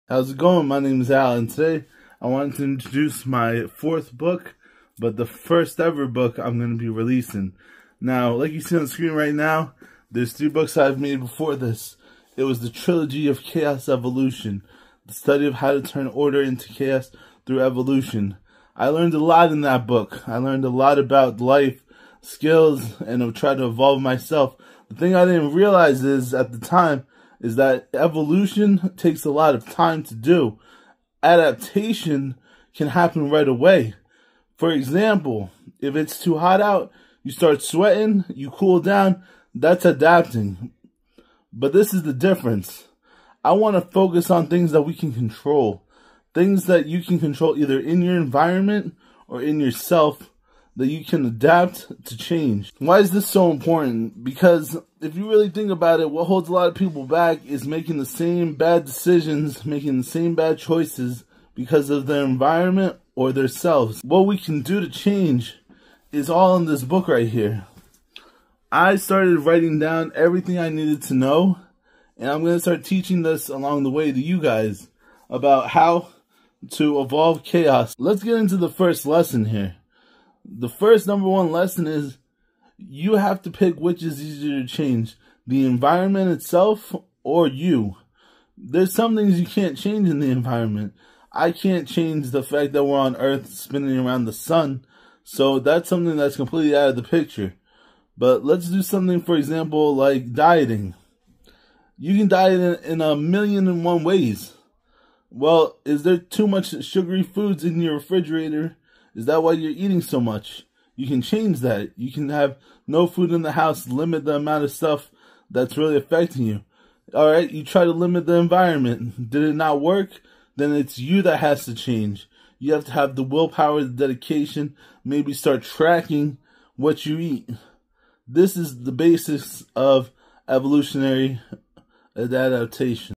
First 3 books will NEVER be released......... but this one will
Chaos evolution was my first 3 part trilogy that will never be released but my latest and greatest evolving choas will be releasing soon to help people